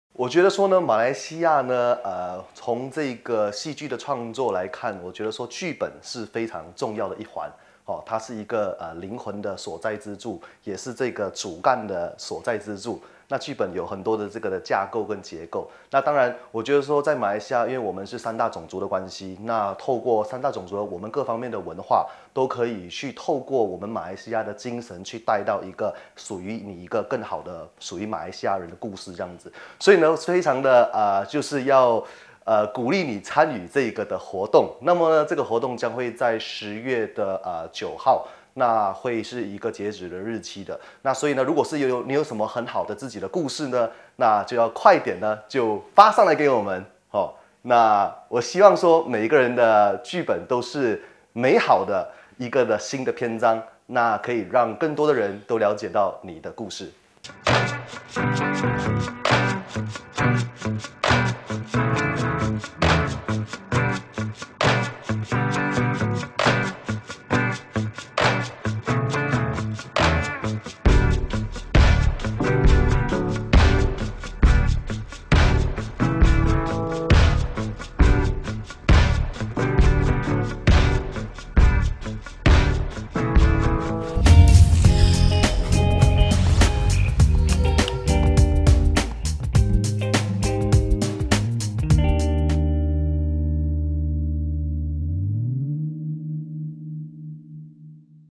《星劇本》宣傳 Kyo庄仲维
Kyo庄仲维呼吁大家踊跃参赛，截稿日期就在10月9号！
欲知更多详情，请浏览星剧本官方网站：
或免费收看星洲电子报《微影视》
请免费下载星洲电子报APP：